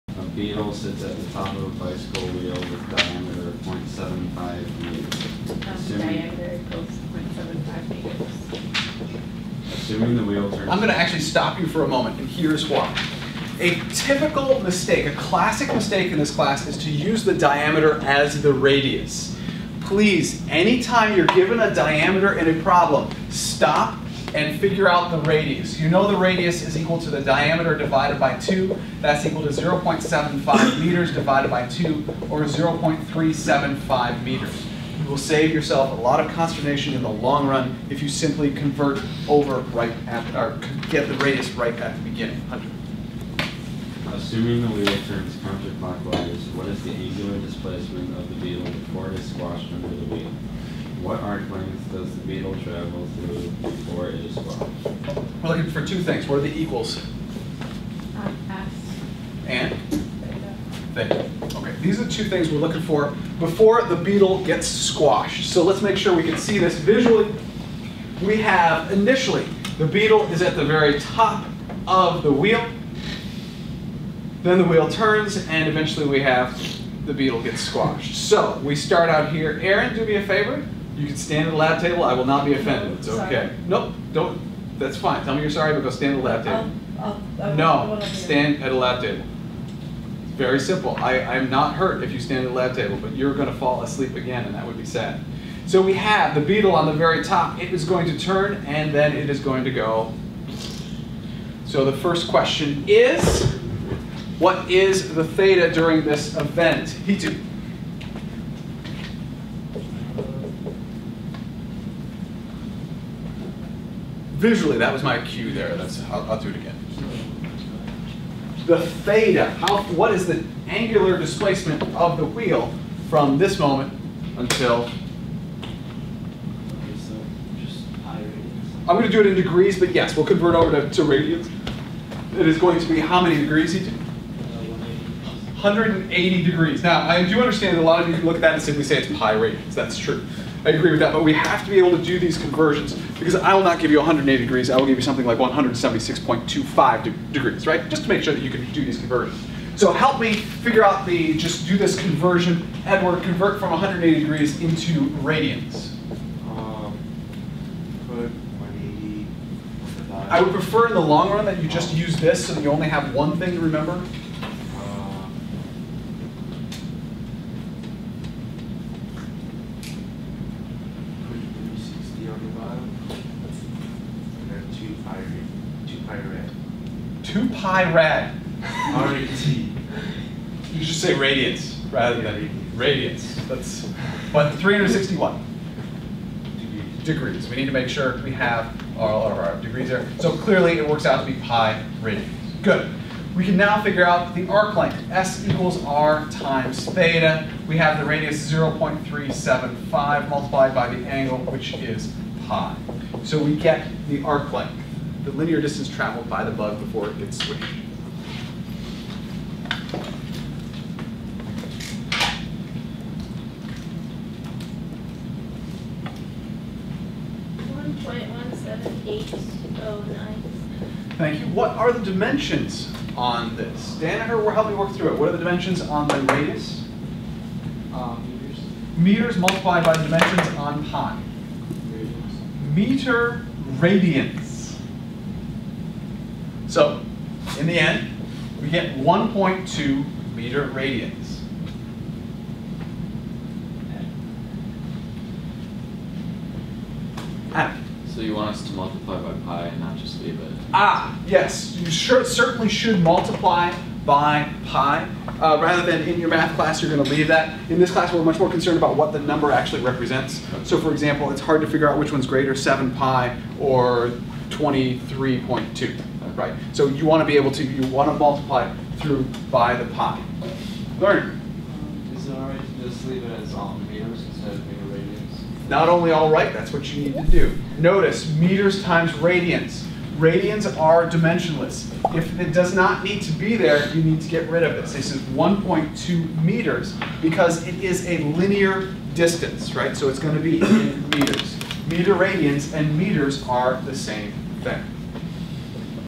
Basic Circular Motion Problem involving Arc Length - Algebra Based Physics
Algebra Based Physics: Basic Circular Motion Problem involving Arc Length. has Lecture Notes, Groupings and Sequencing of my lecture videos. Yes, I changed websites.